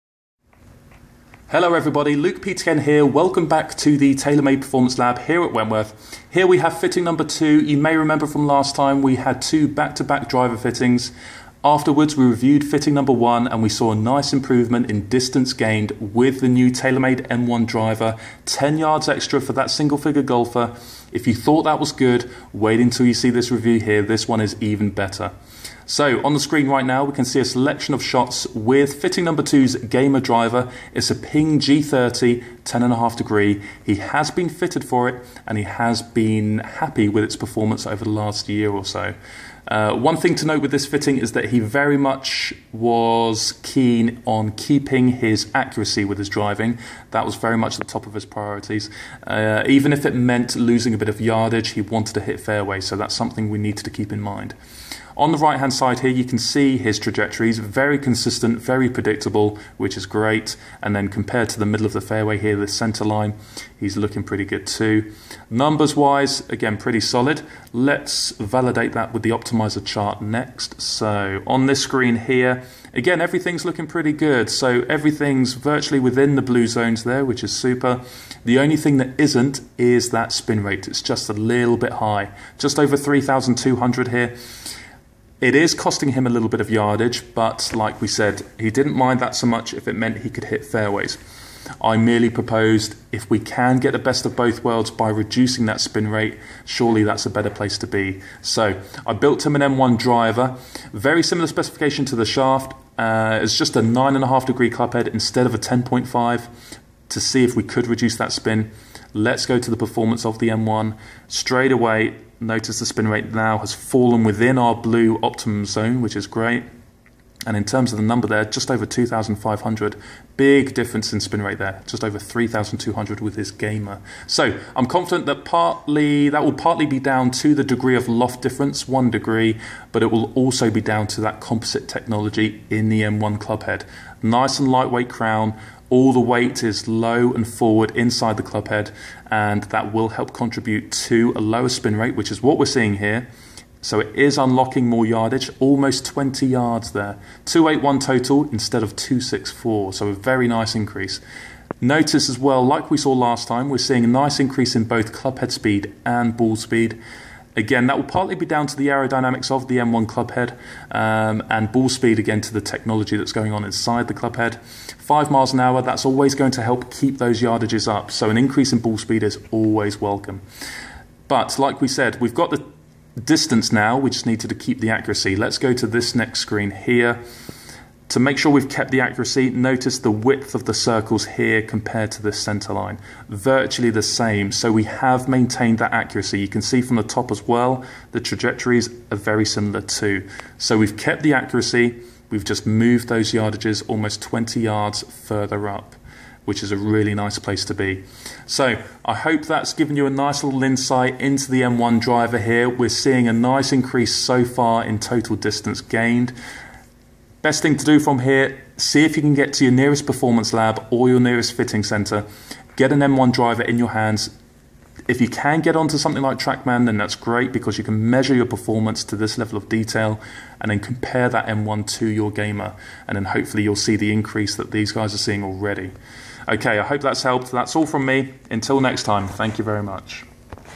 Taylormade M1 driver vs Ping G30!
Really nice change in performance for this single figure handicap golfer with the new M1 driver by Taylormade!